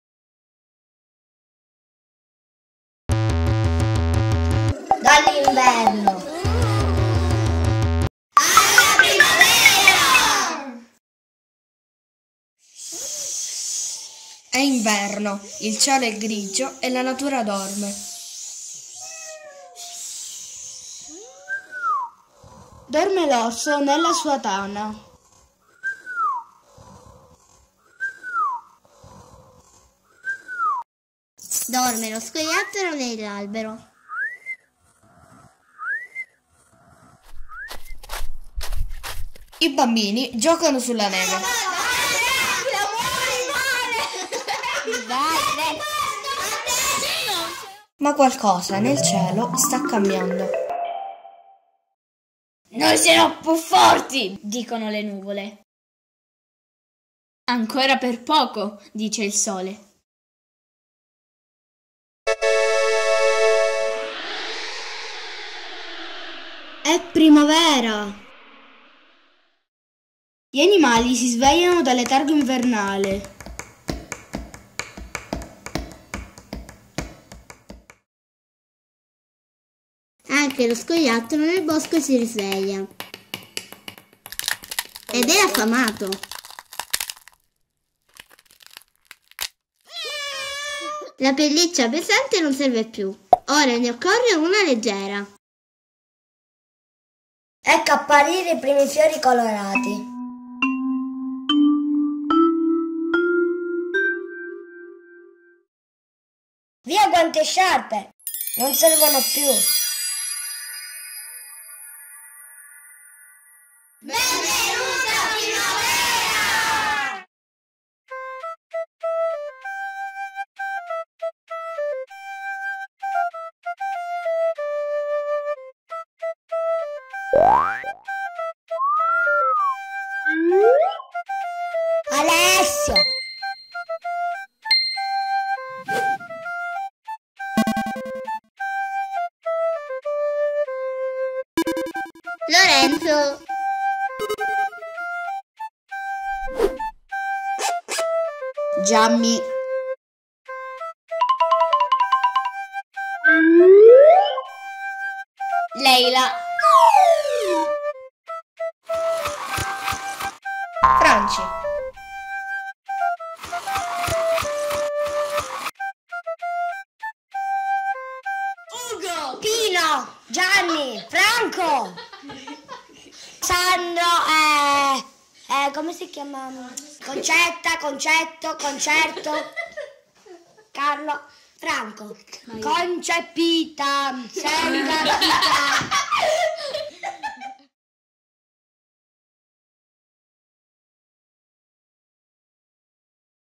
Dall'Inverno alla Primavera
Laboratorio di animazione stop motion con bambini dagli 8 ai 11 anni, realizzato tra Febbraio e Marzo 2018. Serie di scenette rappresentanti il passaggio tra le due stagioni.
Selezionato per l'Epic ACG Fest 2018, festival di animazione ed arti (Albany, San Francisco - California)
Stop-motion animation workshop, held with children aged 8-11 years, made between February and March 2018. A series of little scenes, showing changes in nature along the Winter and Spring transition.